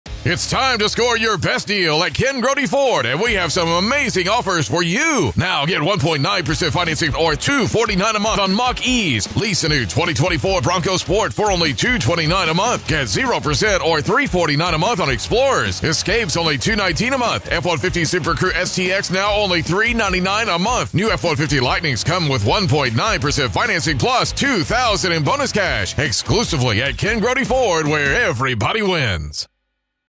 Ken Grody Ford - Score Your Best Deal Sales Event
Lease a New Mustang Mach-E for $249/mo or Lease a New F-150 for $399/mo at Ken Grody Ford during our Score Your Best Deal Sales Event